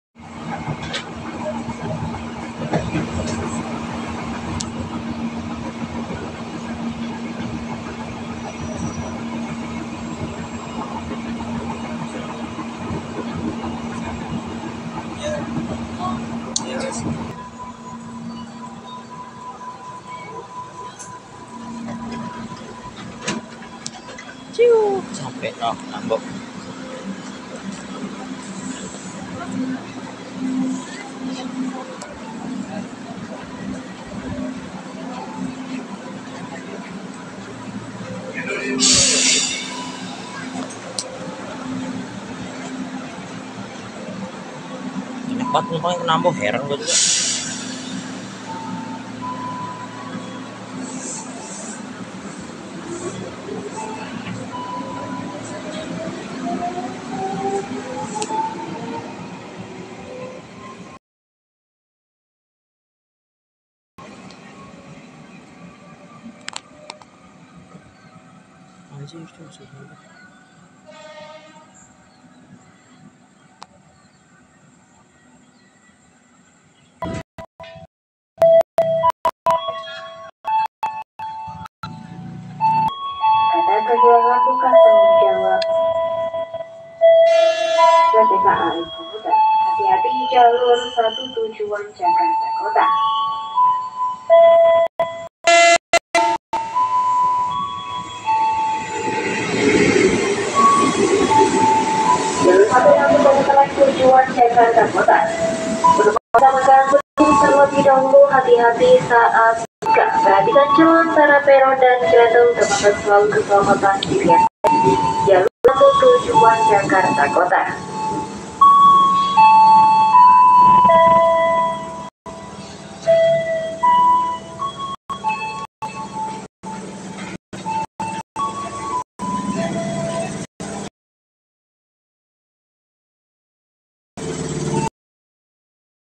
KRL Tokyu 8518 Memasuki stasiun terbaru yaitu PONDOK RAJEG
KRL Tokyu 8518 yang berdinas Relasi Jakarta - Nambo